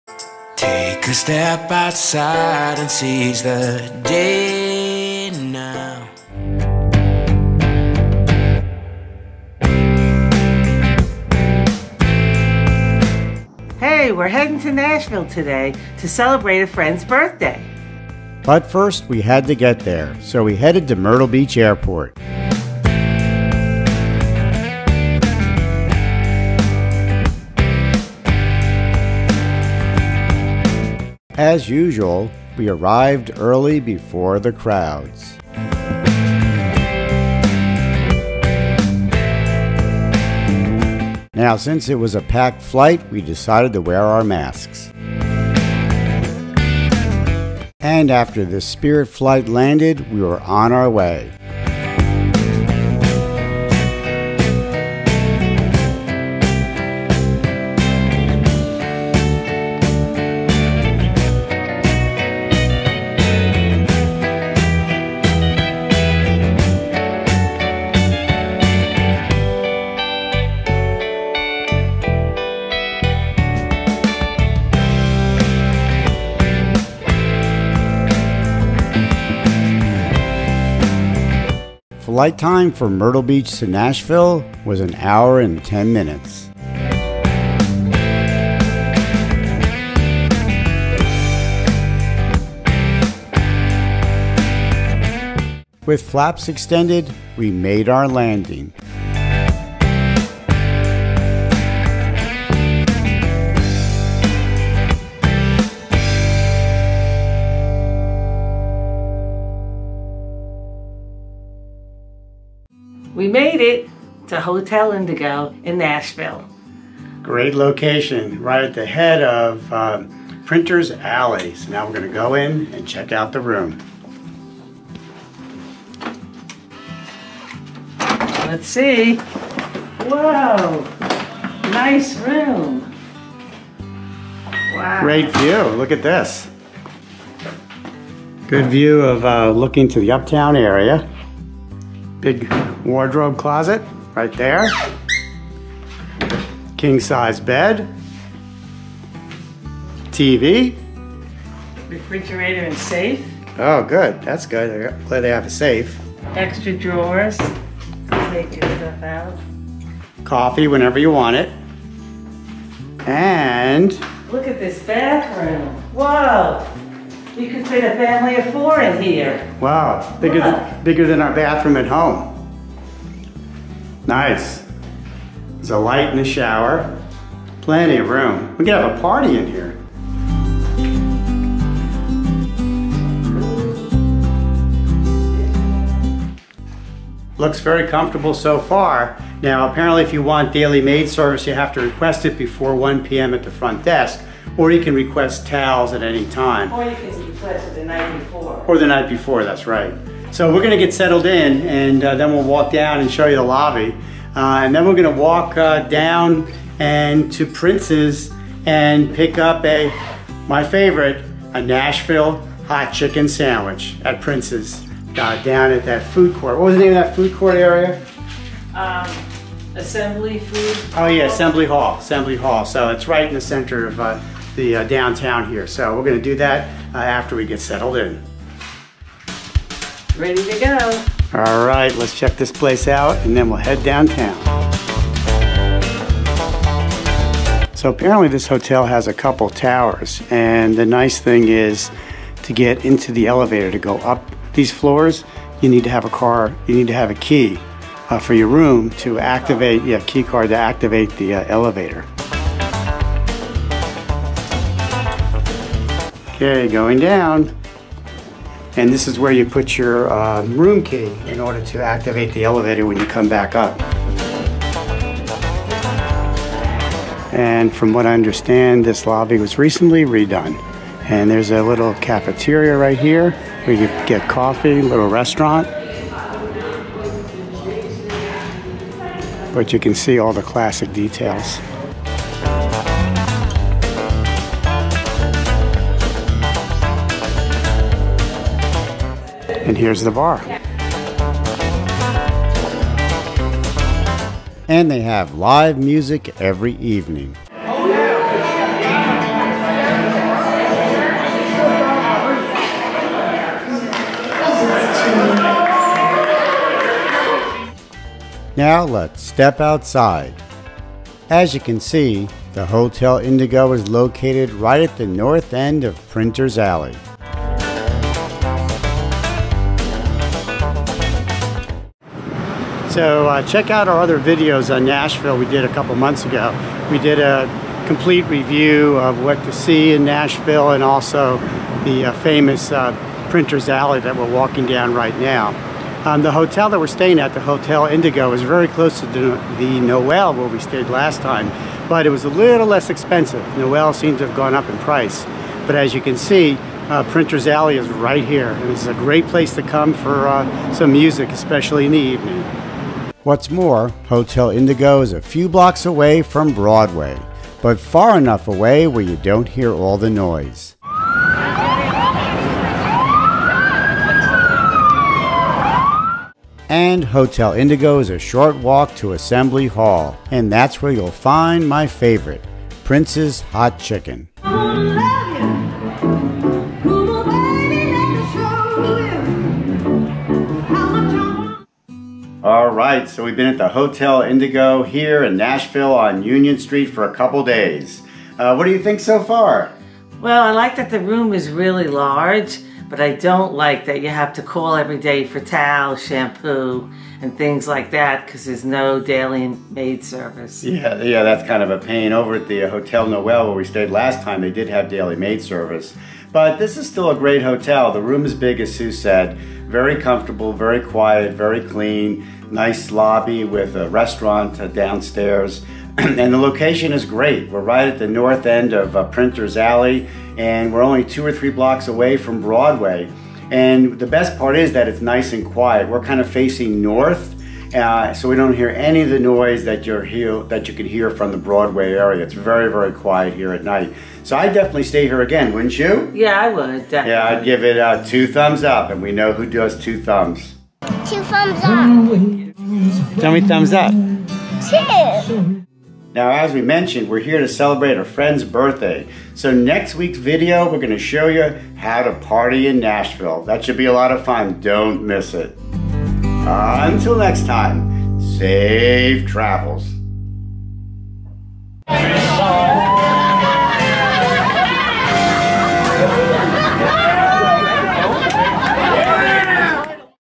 TRAVEL DAY TO NASHVILLE! WE STAY ON PRINTERS ALLEY! Our Hotel Indigo Review (An IHG Hotel)!
We fly Southwest Airlines direct from Myrtle Beach to Nashville for a birthday celebration, and review the Hotel Indigo located at the north end of Printer's Alley! As soon as we arrived, we headed over to Prince's for their hot chicken! No better way to start our visit to Nashville!

Hotel Indigo (An IHG Hotel). Review starts at 1:47 min!